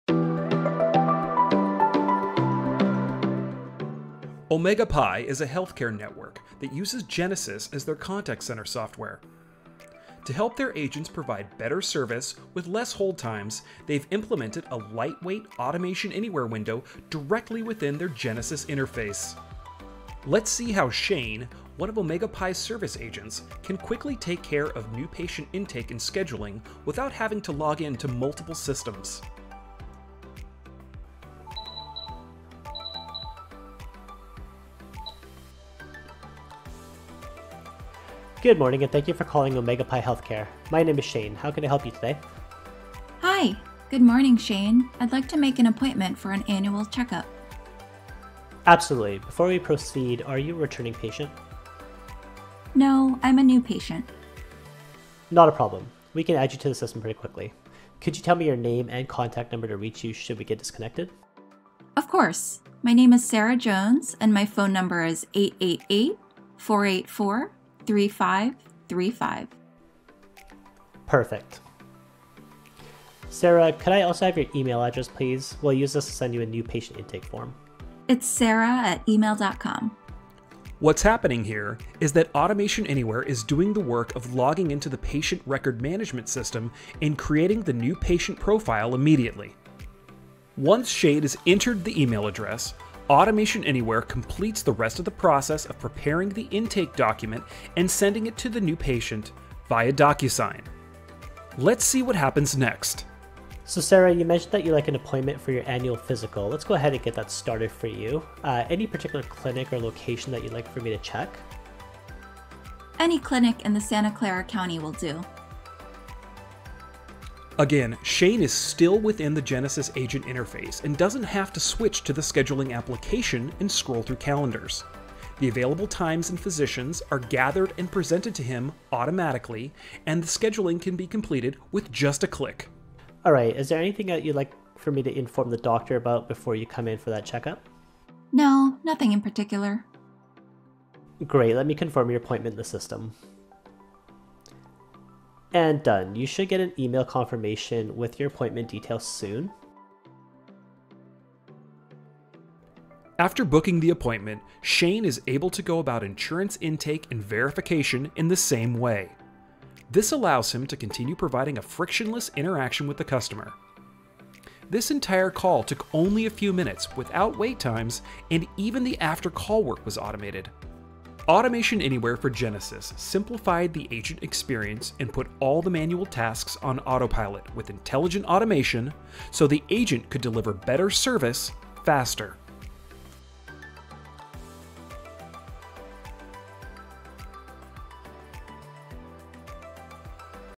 RPA for Genesys Contact Center Software
Automation 360 and Genesys provide a single pane of glass for call center agents in healthcare and other industries. See how the new patient intake process is optimized with intelligent automation, reducing the average call time and helping provide great customer service.